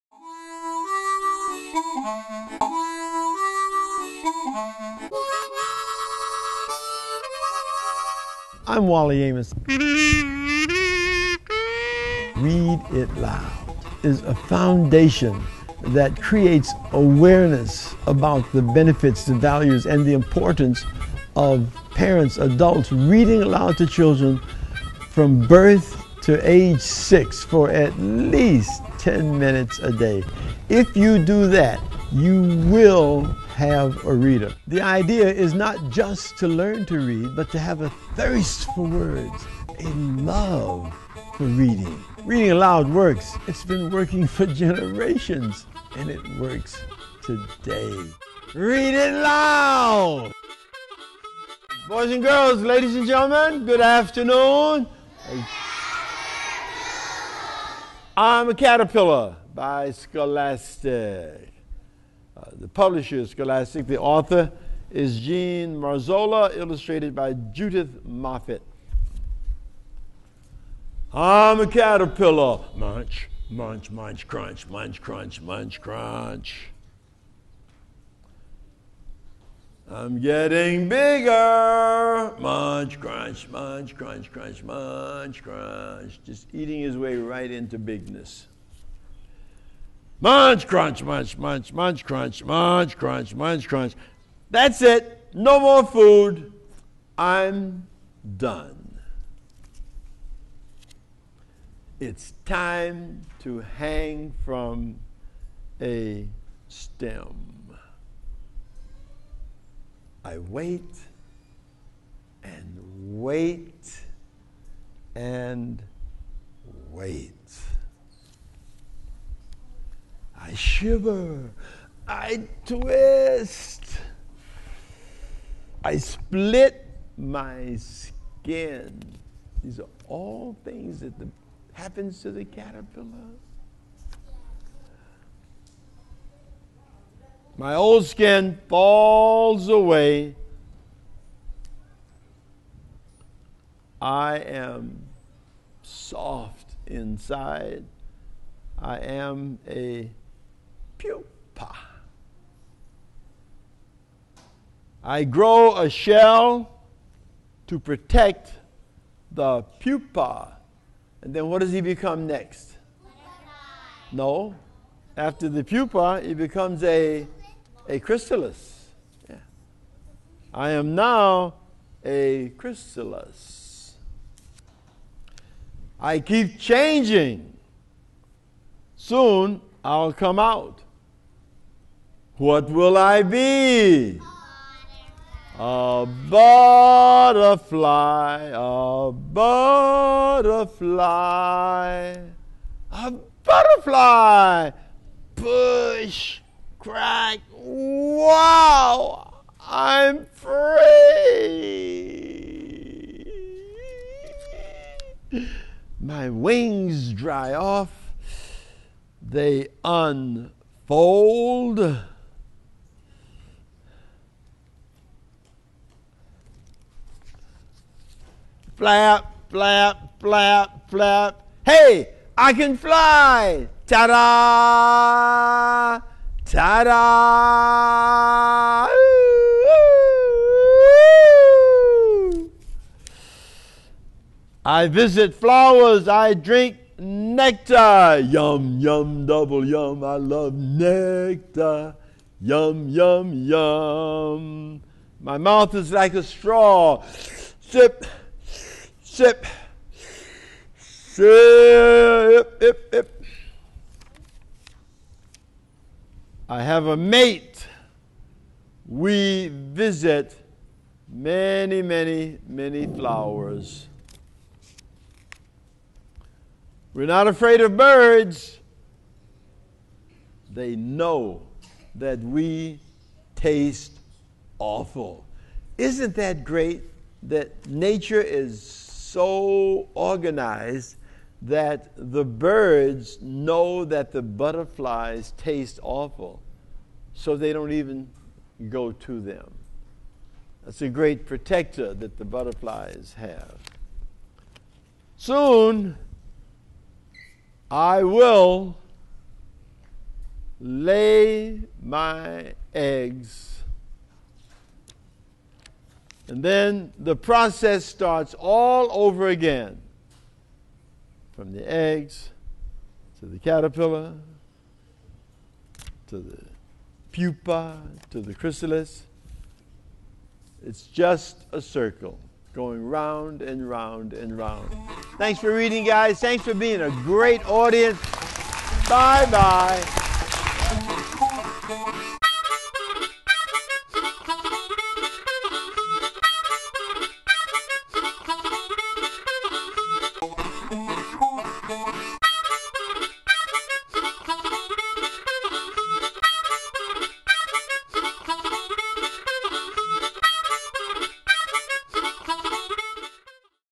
I'm A Caterpillar, by Jean Marzollo
The Read2gether Foundation presents a Wally Amos performance reading.

for more great books and videos.

Moffatt's textured-paper collage illustrations and Marzollo's easy text bring this beginning reader to life. The story presents just enough facts about the life cycle of a caterpillar to interest young students. The extra-large print and short sentences will enable first graders to read it alone. It is told in the caterpillar's light voice, "I visit flowers. I drink nectar. Yum!".